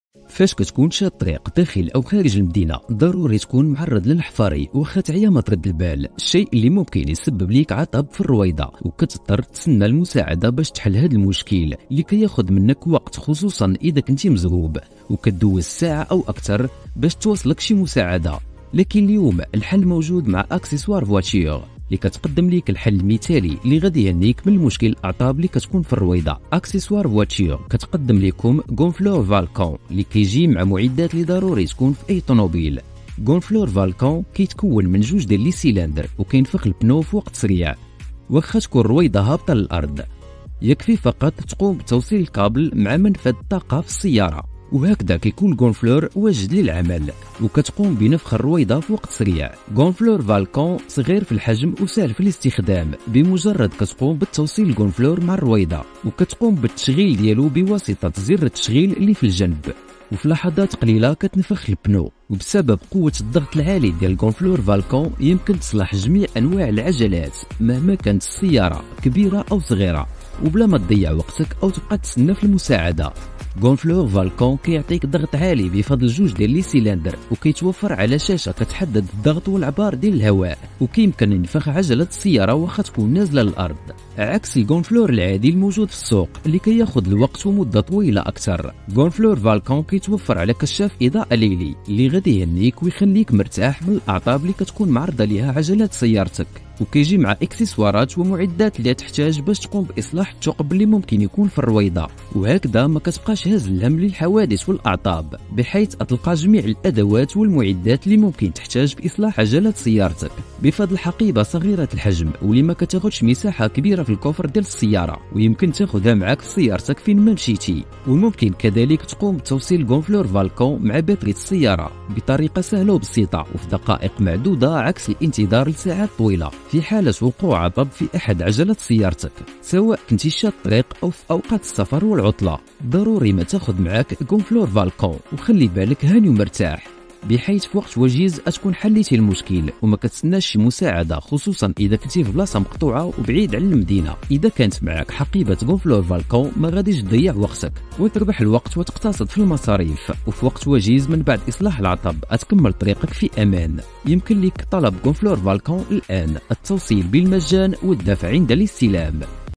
Gonfleur pneu 2 cylindres
Pompe gonflable voiture 2 cylindre 150 PSI avec boîte à outils
Pompe à Air pour cylindre, 150W, pour Automobile, boîte à outils prix pas cher sur accessoirevoiture.ma